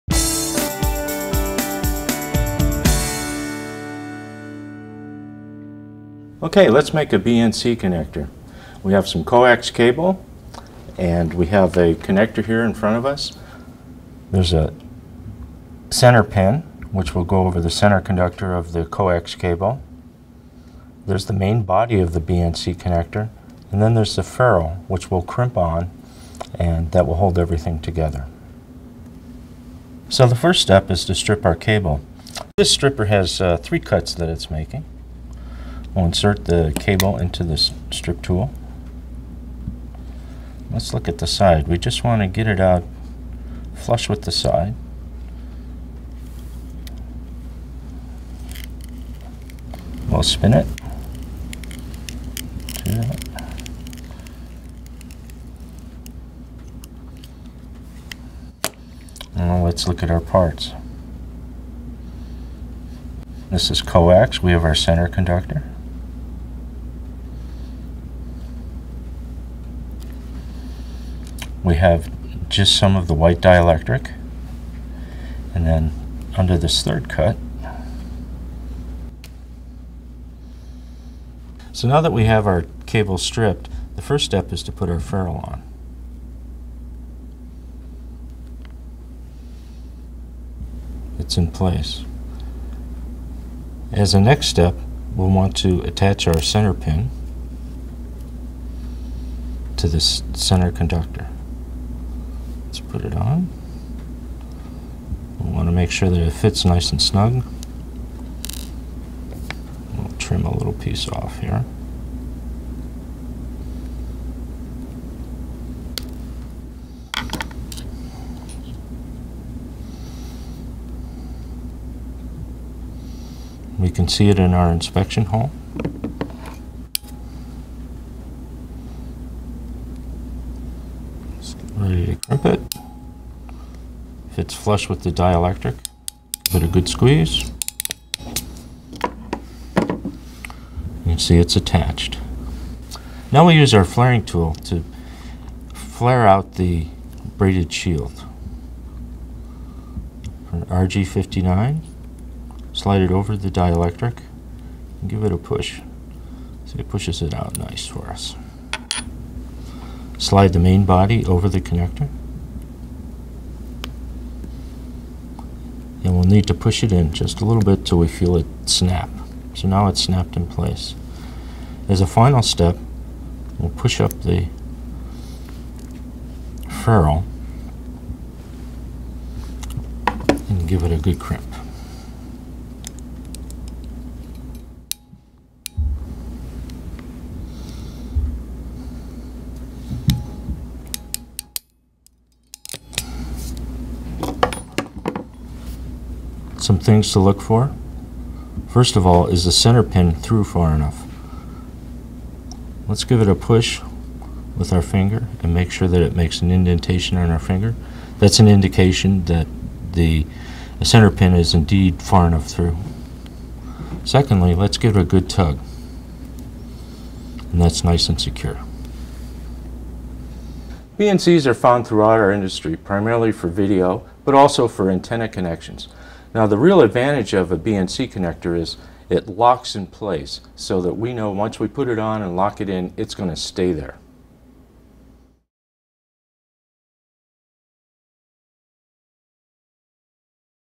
Crimping BNCs onto RG59
Learn how to crimp a BNC connector onto an RG59 coax cable with InfoComm University instructor Bill Thomas, CTS-I.